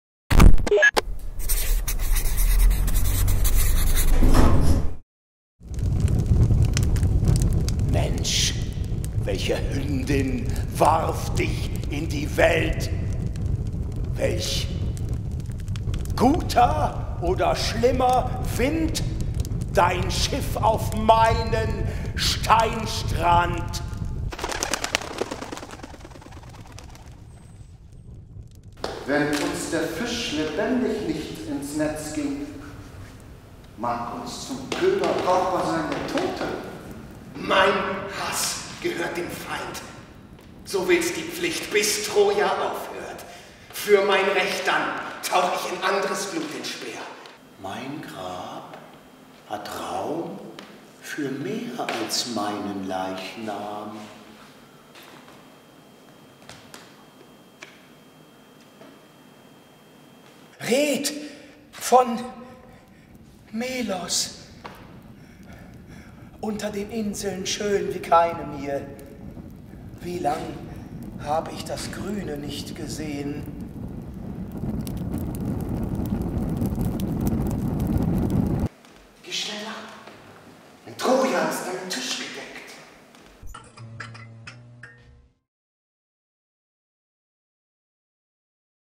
»Philoktet« Trailer
»Philoktet«
Heiner Müller
Regie Elias Perrig
Bühne und Kostüme Beate Faßnacht
Dramaturgie Verena von Waldow
Mit Carsten Hentrich, Andreas Jeßing, Moritz Schulze